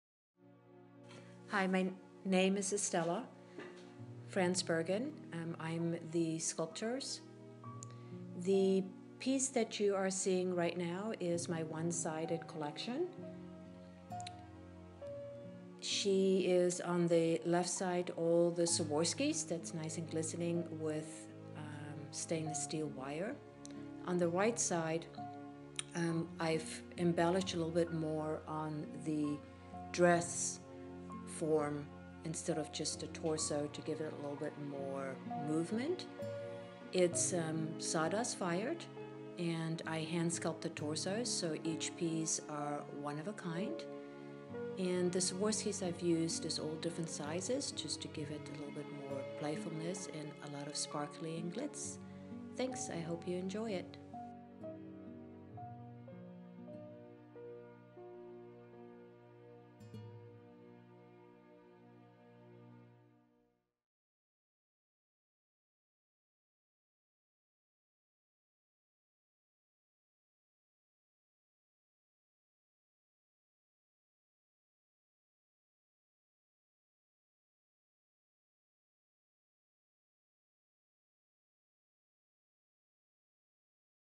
Asymmetrical sculpture by Estella Fransbergen
Clay sculpted torso with asymmetrical long dress.
Sawdust fired, embellished with swarovski's